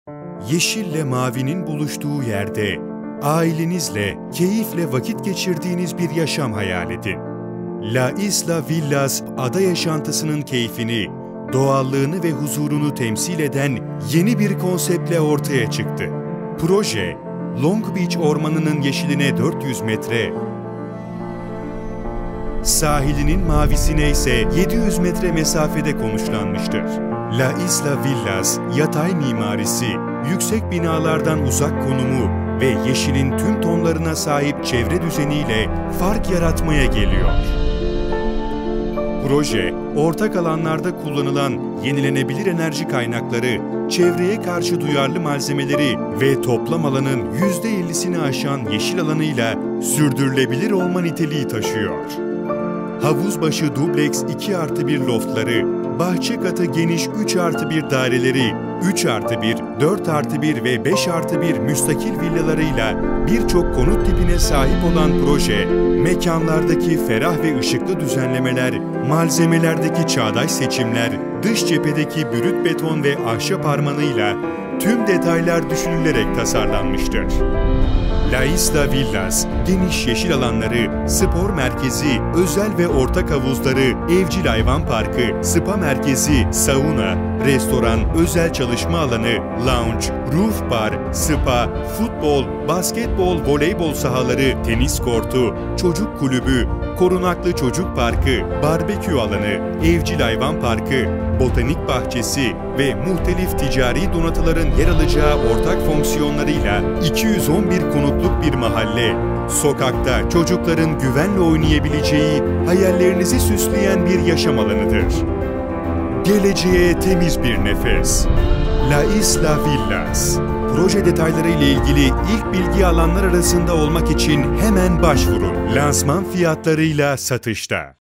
Geleceğe Temiz Bir Nefes ''LA ISLA Villas'' Lansman Fiyatlarıyla Satışta!
Proje Detayları ile İlgili Bilgi Almak için Linke Tıklayın.

La Isla Villas'ta Avantajla Dolu Ön Talep Süreci Başladı.
Lansman Fiyatları ve Proje Detayları ile İlgili İlk Bilgi Alanlar arasında Olmak için Hemen Başvurun.
Yeşil ile mavinin buluştuğu yerde,
Ailenizle Keyifle Vakit Geçireceğiniz Bir yaşam hayal edin

LA ISLA Villas ada yaşantısının keyfini, doğallığını ve huzurunu temsil eden yeni bir konseptle ortaya çıktı!
Proje Long Beach Ormanının Yeşiline 400 metre, Sahilinin mavisine ise 700 metre mesafede konuşlanmıştır.
La Isla Villas Yatay mimarisi, yüksek binalardan uzak konumu ve yeşilin tüm tonlarına sahip çevre düzeni ile fark yaratmaya geliyor. Proje, ortak alanlarda kullanılan yenilenebilir enerji kaynakları, çevreye karşı duyarlı malzemeleri  ve toplam alanın %50’sini aşan yeşil alanı ile sürdürülebilir olma niteliği taşıyor.
Havuz başı dubleks 2+1 loftları, bahçe katı geniş 3+1 daireleri, 3+1, 4+1 ve 5+1 Müstakil villaları ile bir çok konut tipine sahip olan proje, mekanlardaki ferah ve ışıklı düzenlemeler, malzemelerdeki çağdaş seçimler, dış cephedeki brüt beton ve ahşap harmanı ile tüm detaylar düşünülerek tasarlanmıştır.
Geleceğe temiz bir Nefes! La Isla Villas!
Lansman Fiyatları ile Satışta.